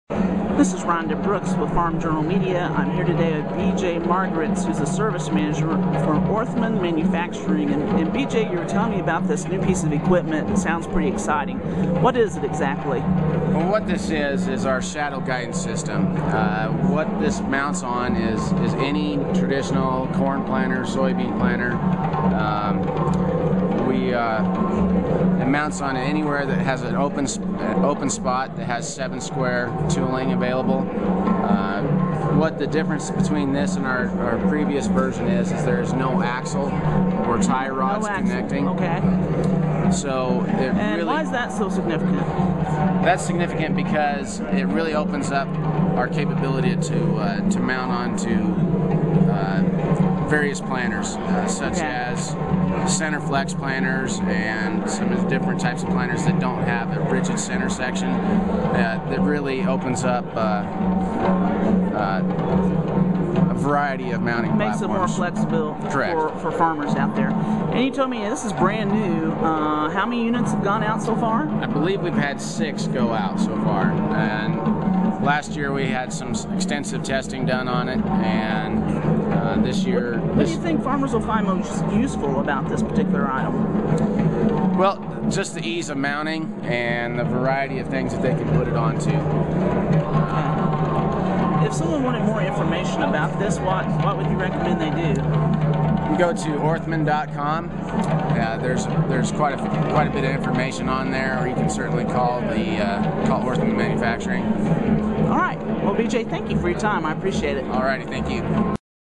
GPS Shadow Tracker
Just in time for planting this spring, Orthman Manufacturing is introducing its new GPS Shadow Tracker. "The system opens up our capability to mount on various types of planters, like those that don't have a rigid center section," says B. J. Margritz, service manager for the company.